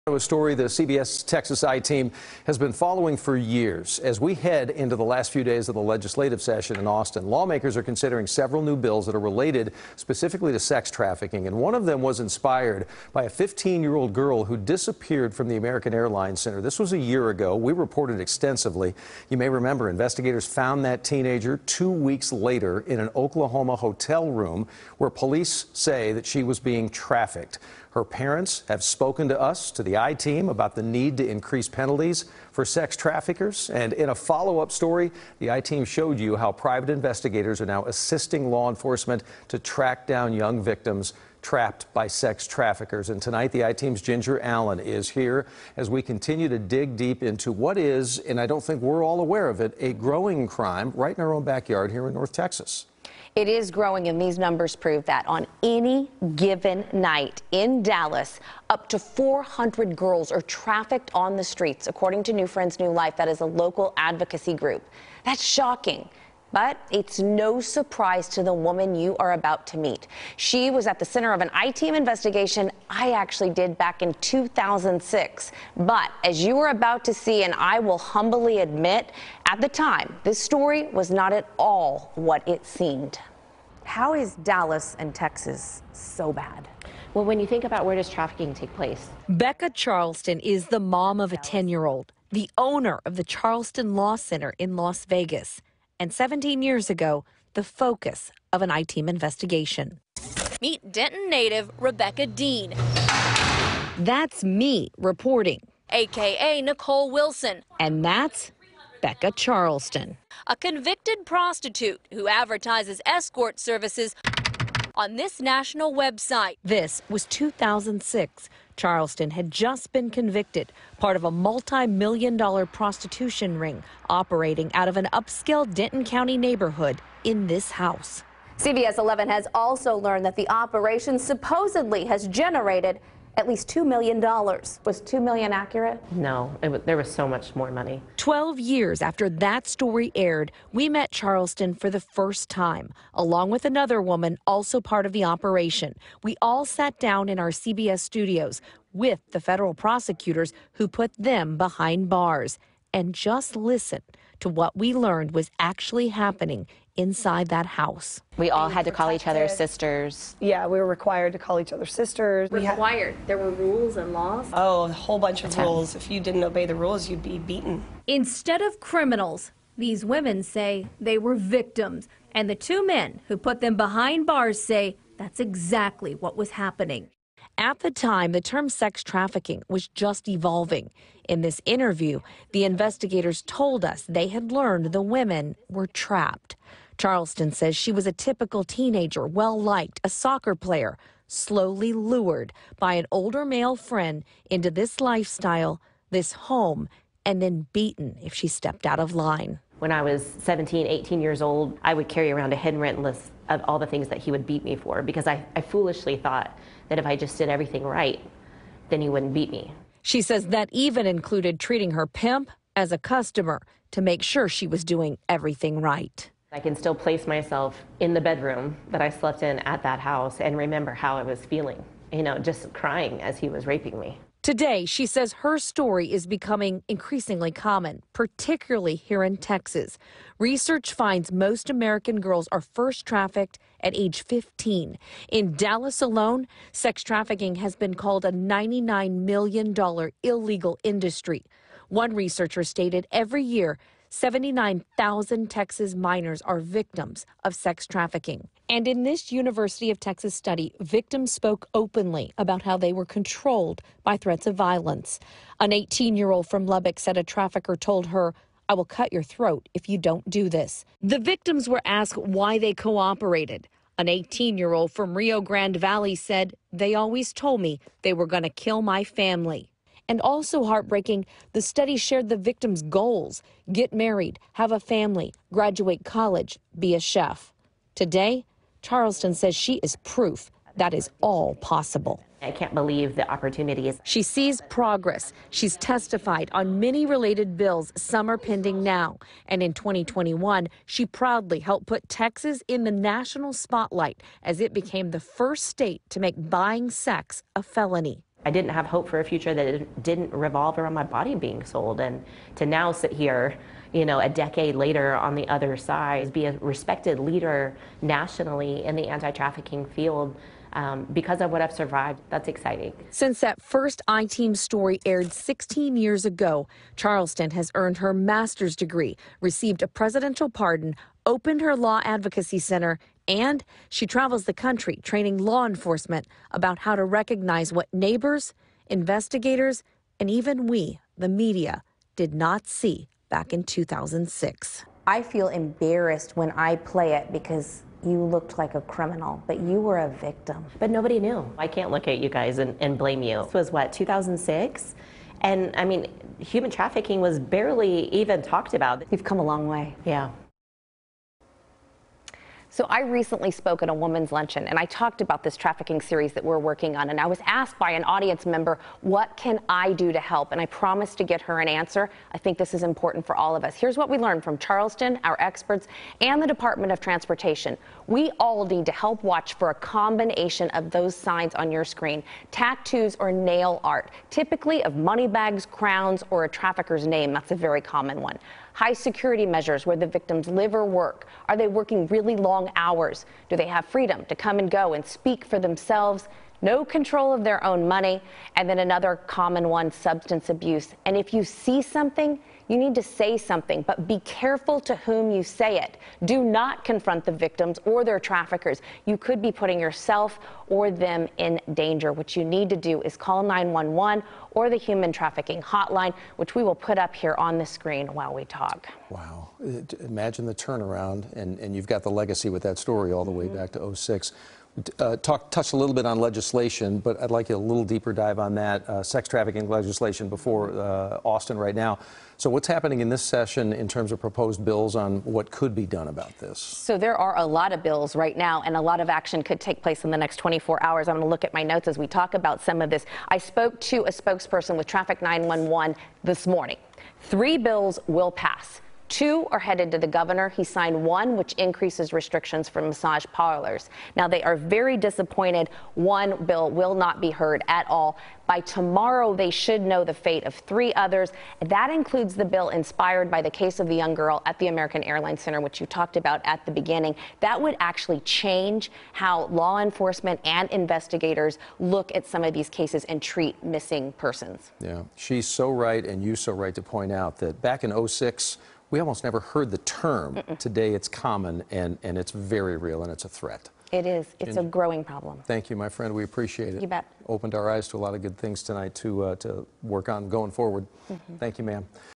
Sex trafficking in Texas: "I foolishly thought if I did everything right, he wouldn't beat me again"
On any given night in Dallas, 400 girls are trafficked on the streets according to New Friends New Life, a North Texas human trafficking advocacy group.